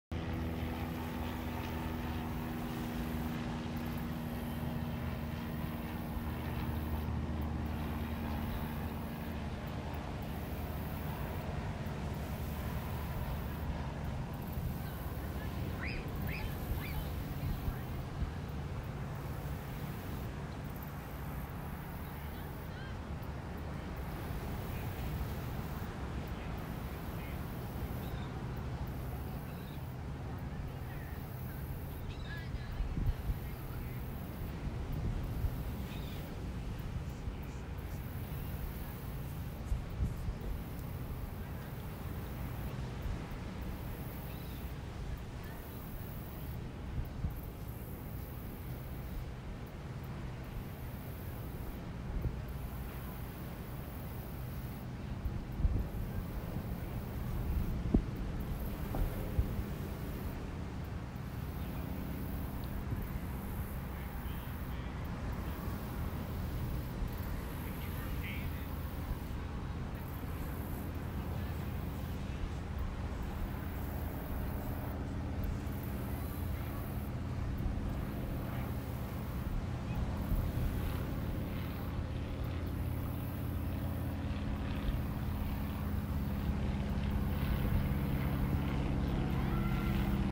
Your OBX Getaway
Come on to the Outer Banks and relax with us on some of the best beaches on the east coast!

Joe Lamb Jr. & Associates